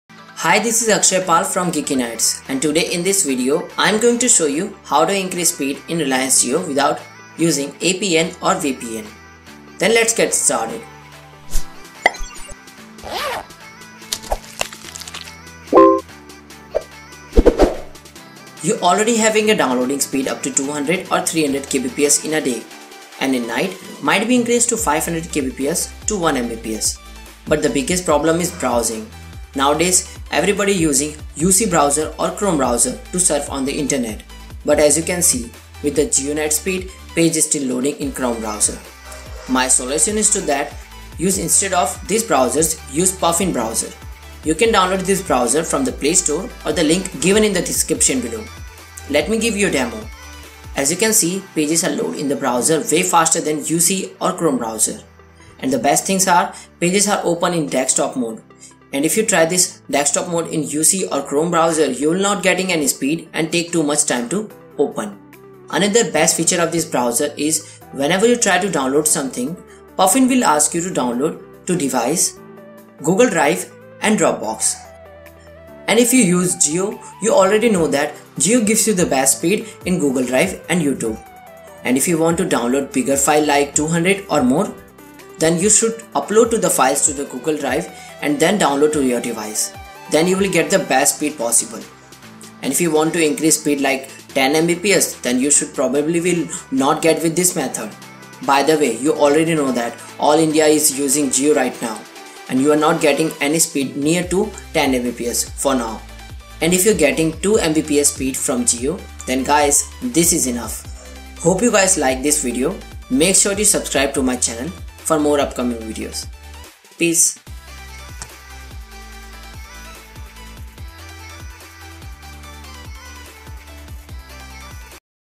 Best trick ever for jio users How to increase Reliance Jio Speed☺ [Browsing & downloading]
in this video i have shown how to increase reliance jio browsing and downloading speed by some basic tips. As you all are not getting jio speed and now there is only 1gb cap per day, so follow the steps provided in this video to get High speed reliance jio 4g network for browsing and downloading.How to increase reliance jio speed to a large extent. How to increase reliance jio internet speed. Increase jio speed beyond your imagination.
* For increasing browsing speed or experience on you Reliance jio you must use Puffin Browser. It gives you a Best expirience you ever get on Reliance Jio.
* For increasing download speed also use Puffin Browser because with puffin browser you can directly upload your files to your Google Drive or Dropbox and from there you download your files with a high speed of Relince jio 4G.
* sometimes you have to ON the airplane mode to get the Powers of Reliance Jio Back.
* If you want to get More than 1GB jio Data per day
Then simply Get Another Jio Sim.